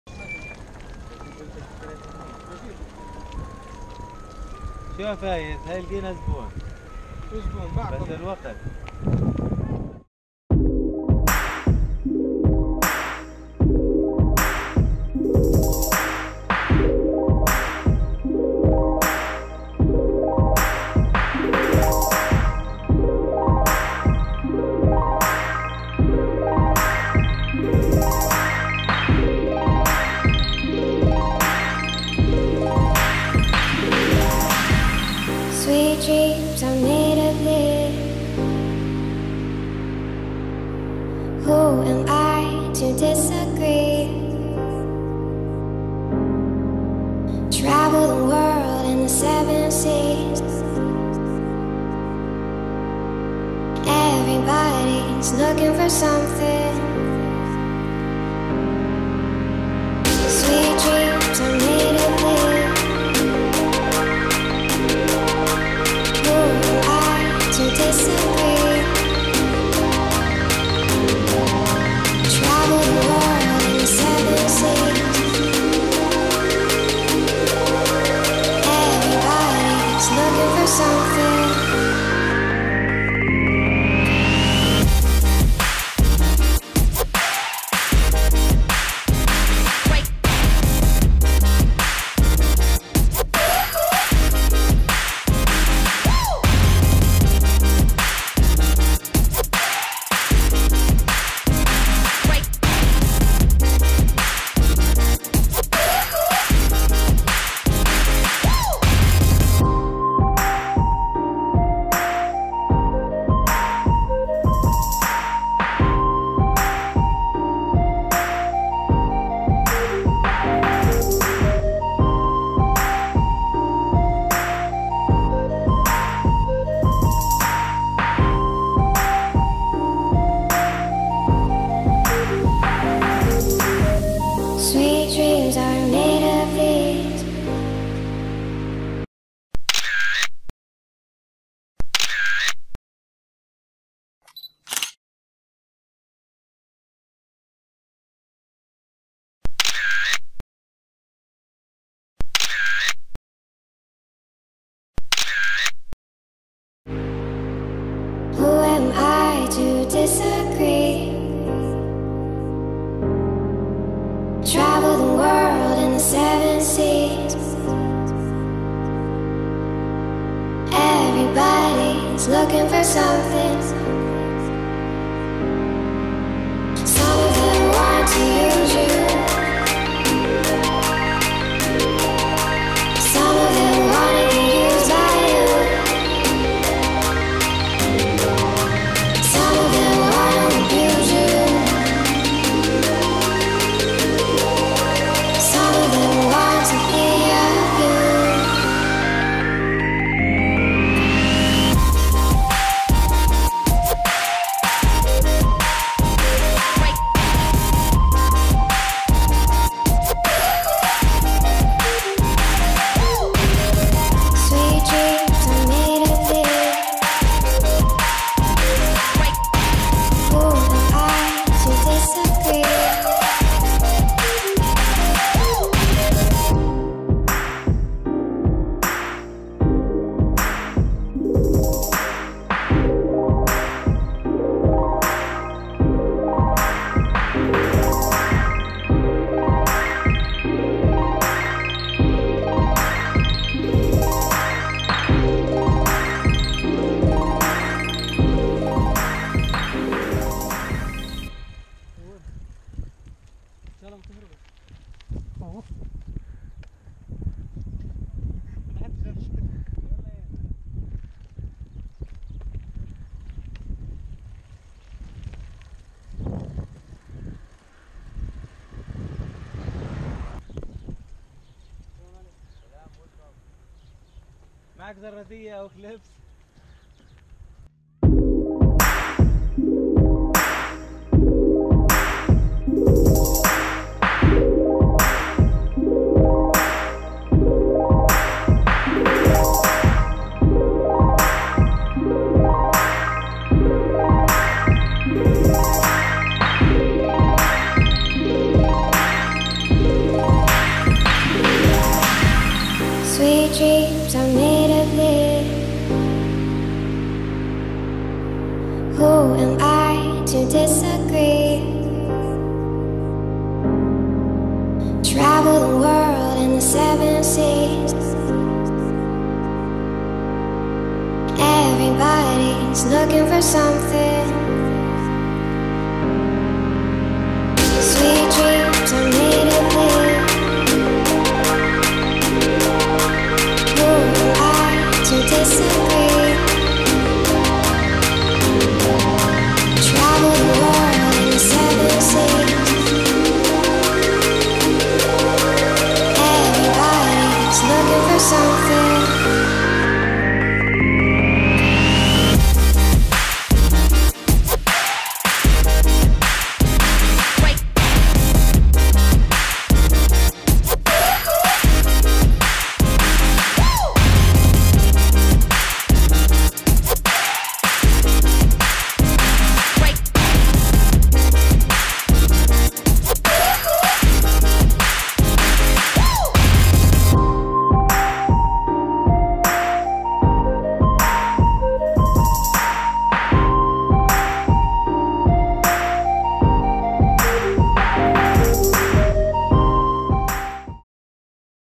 Al-Auja Valley Bike+Hike Trip VLOG واد العوجا بالدراجات
Bike trip from Ramallah to Al-Auja valley then Jericho (58 KM).
March 2018